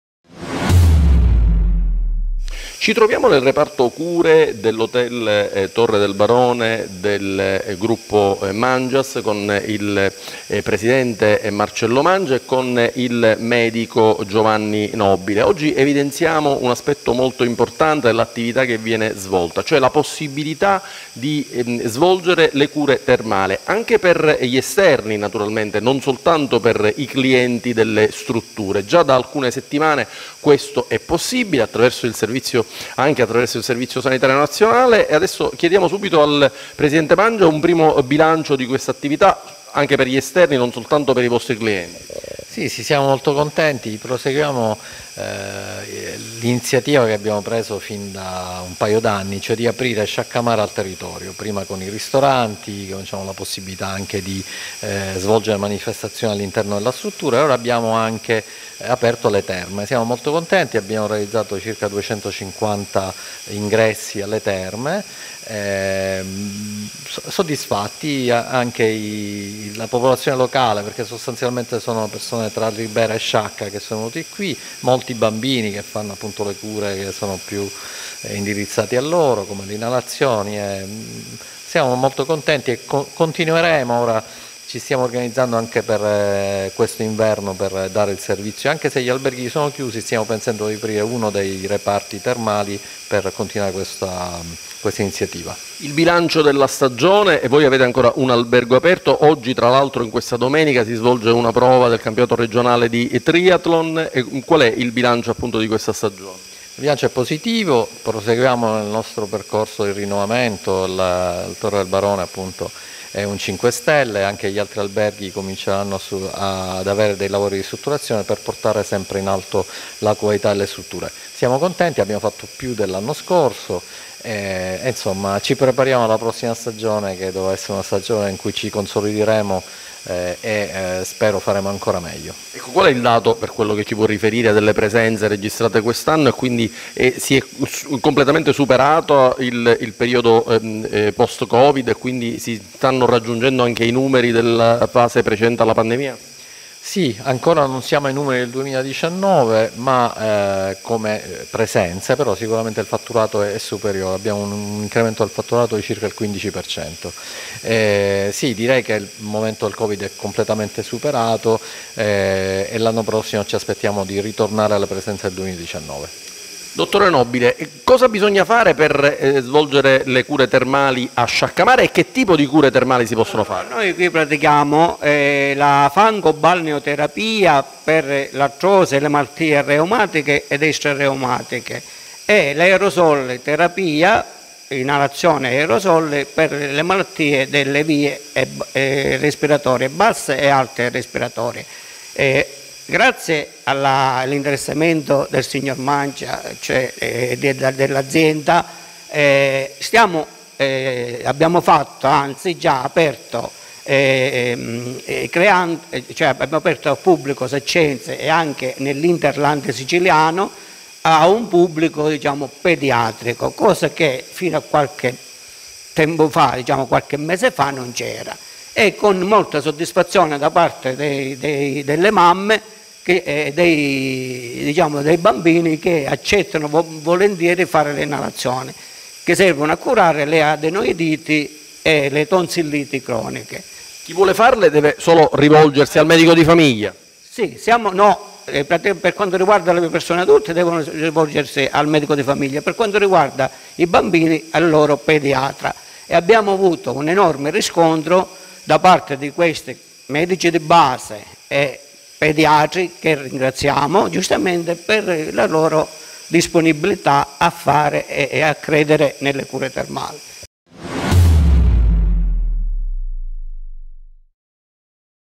Cure termali a Sciaccamare
Positivo a Sciaccamare il bilancio delle cure termali aperte agli esterni alle strutture alberghiere. Sono stati 250 i saccensi e tanti arrivati anche dai Comuni della zona che, pur non essendo ospiti delle strutture alberghiere, hanno potuto svolgere le cure termali prescritte da medici di famiglia e pediatri e dunque beneficiando del servizio sanitario nazionale.